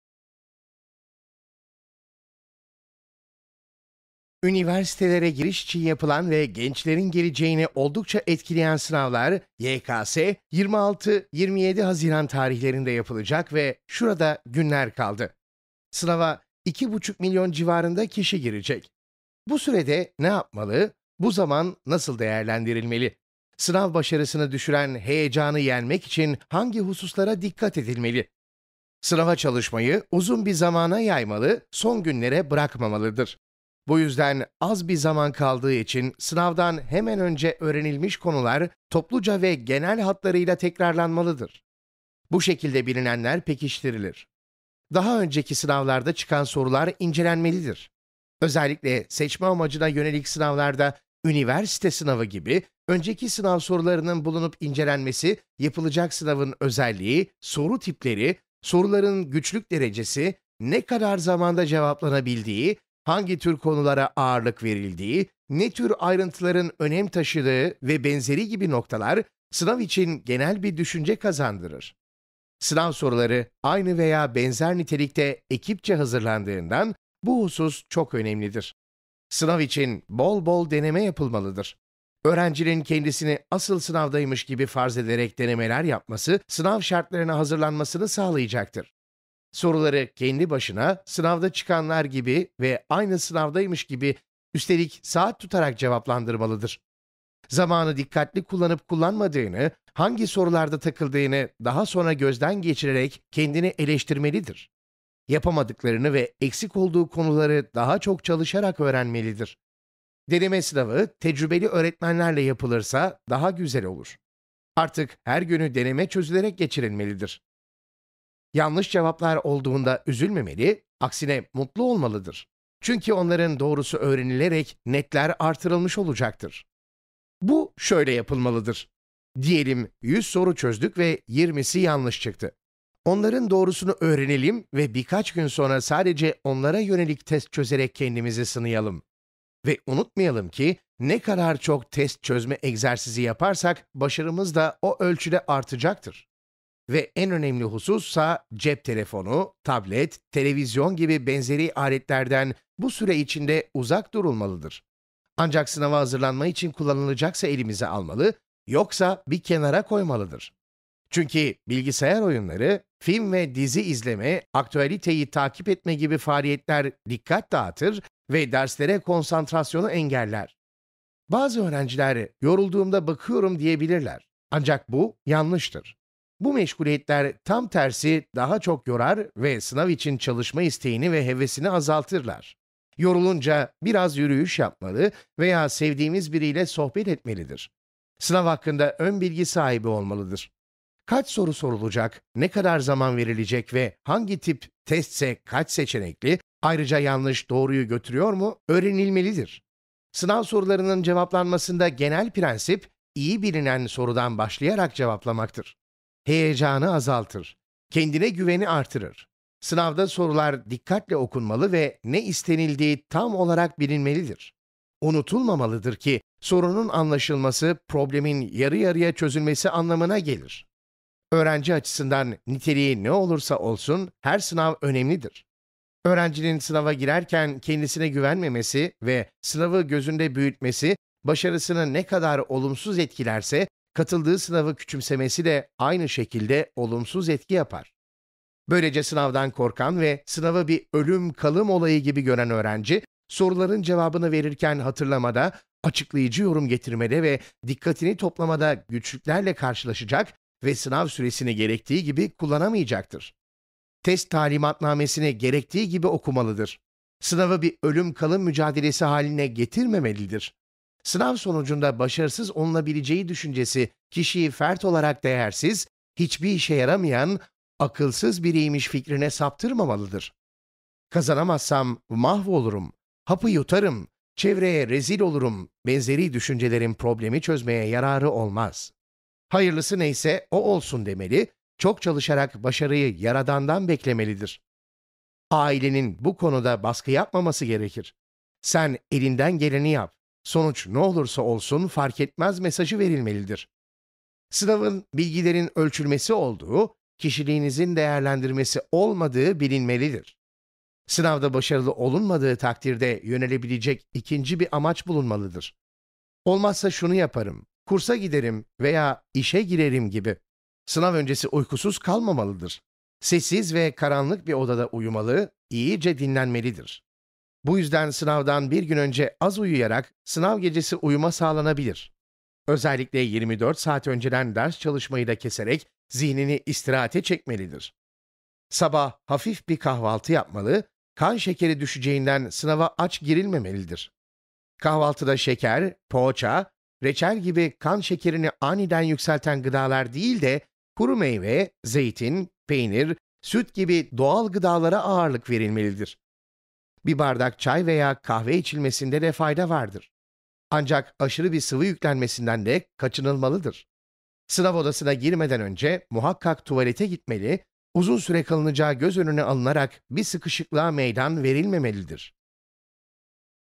Üniversite Sınavına Gençler Nasıl Hazırlanmalı? - Sefa Saygılı
Üniversite sınavına sayılı günler kala gençler üniversite sınavına nasıl hazırlanmalı?